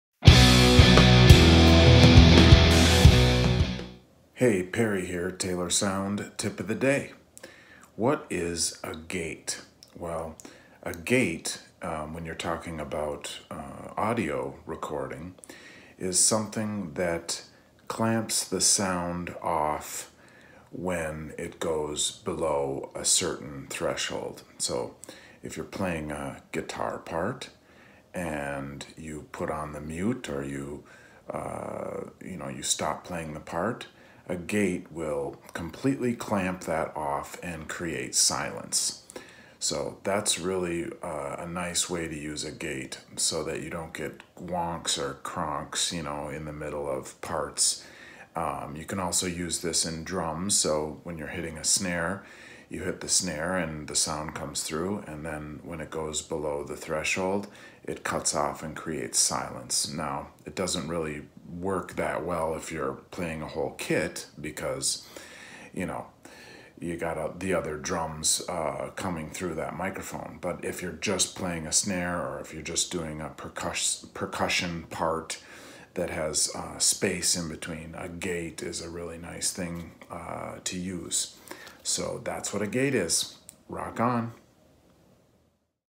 Mixing Terms - What is a Gate?
When you are mixing a song when do you want to use a gate?